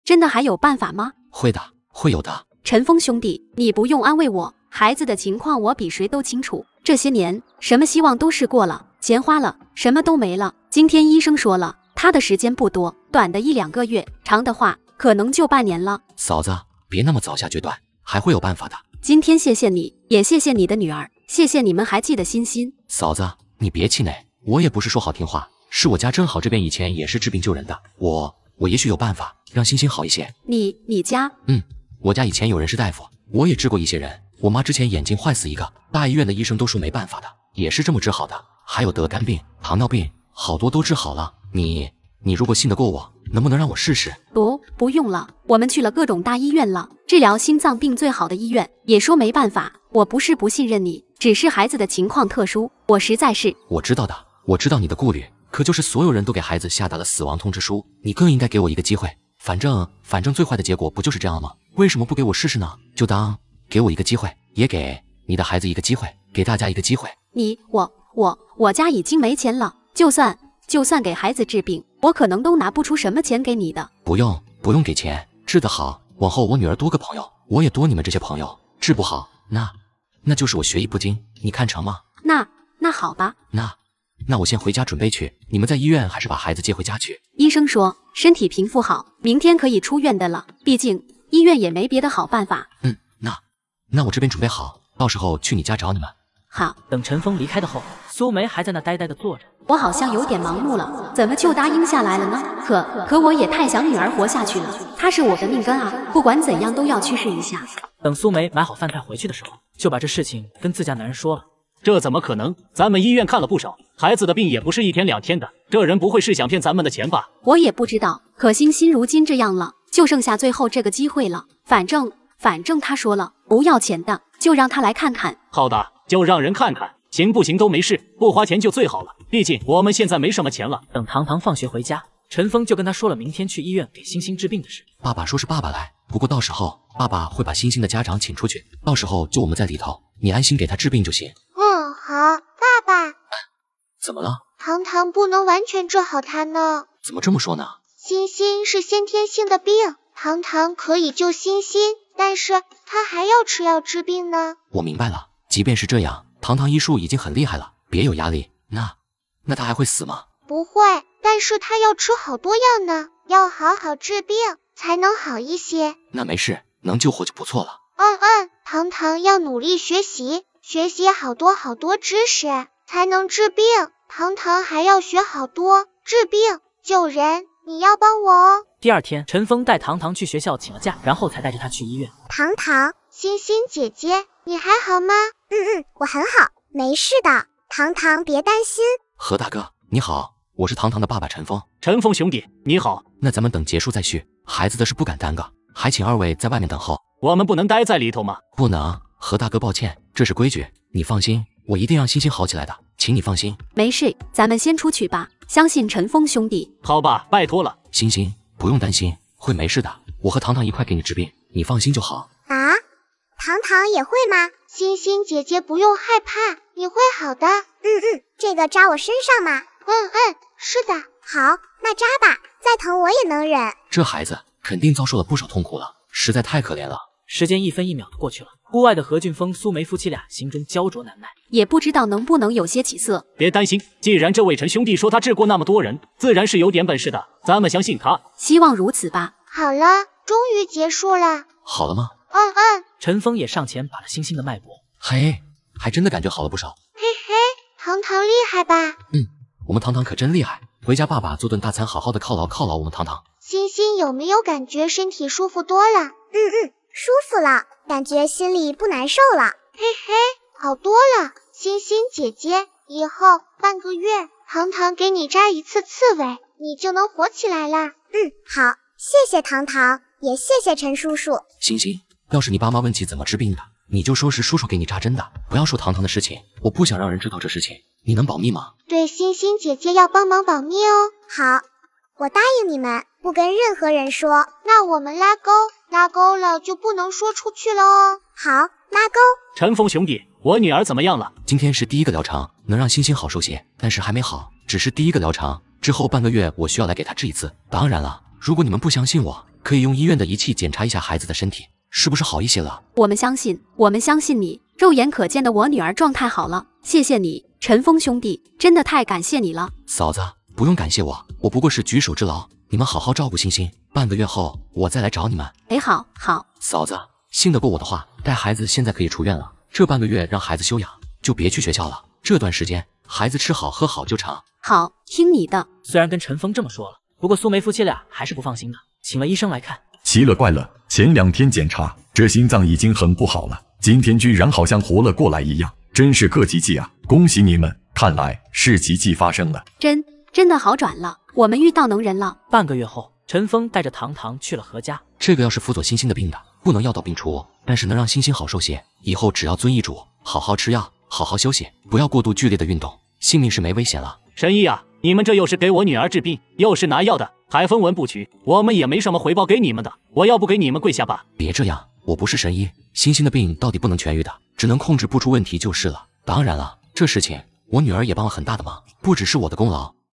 【萌娃糖糖】EP 272丨糖糖去給鑫鑫治病，之後鑫鑫拍片檢查，竟真的發生了好轉！ 【阿飛】
糖糖去給鑫鑫治病，之後鑫鑫拍片檢查，竟真的發生了好轉！

大家好我是阿飛
歡迎大家來看我的沙雕動畫
也很高興能給大家帶來樂趣